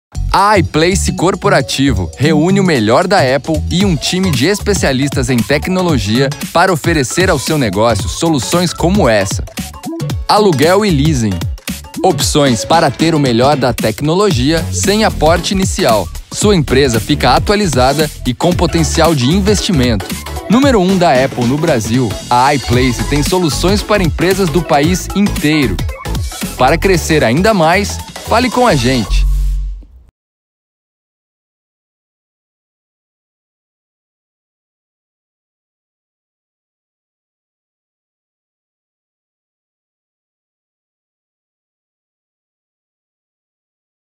Apple para Empresas - Leasing Operacional - iPlace
Uma das principais vantagens do leasing operacional é que o cliente tem a opção de adquirir em definitivo os dispositivos após o período de arrendamento. Para tanto, basta pagar o valor residual ao término do contrato. Alternativamente, é possível optar pela renovação do leasing, atualizando os equipamentos para versões mais modernas.